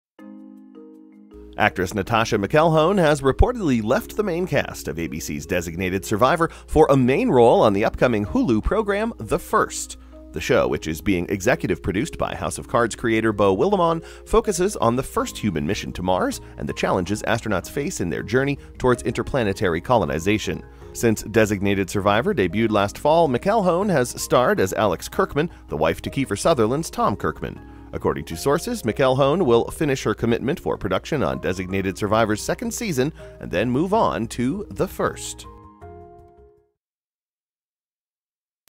Natascha McElhone to Leave ABC’s ‘Designated Survivor’
Actress Natascha McElhone has reportedly left the main cast of ABC's 'Designated Survivor' for a main role on the upcoming Hulu program 'The First'. The show, which is being executive produced by by “House of Cards” creator Beau Willimon, focuses on the first human mission to Mars and the challenges astronauts face in their journey towards interplanetary colonization. Since 'Designated Survivor' debuted last fall, McElhone has starred as Alex Kirkman, the wife to Kiefer Sutherland's Tom Kirkman. According to sources, McElhone will finish her commitment for production on “Designated Survivor's' second season and then move on to “The First."